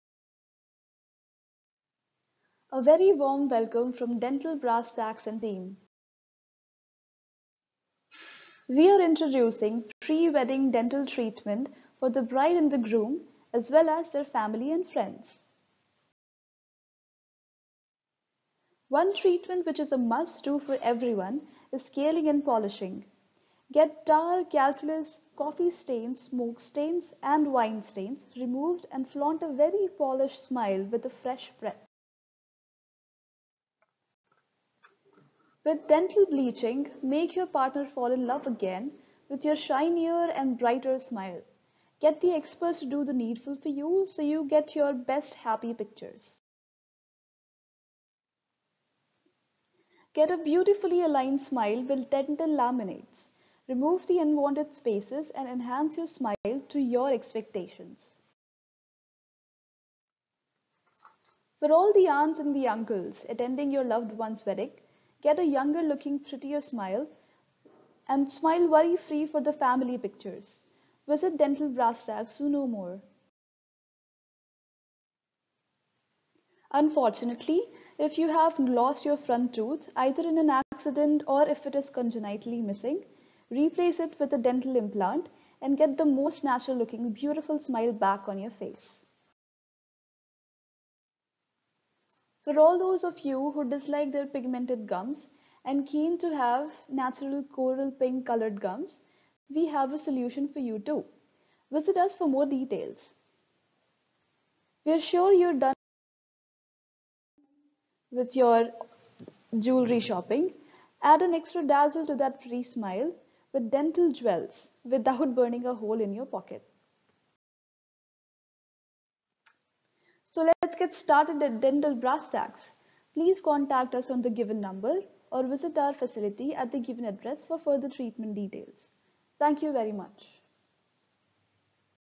PRE-WEDDING DENTAL TREATMENTS
INTRODUCING PRE-WEDDING /PRE-BRIDAL DENTAL TREATMENT OPTIONS AT DENTAL BRASSTACKS, MULTISPECIALITY DENTAL CLINIC AT SOHNA ROAD, GURGAON.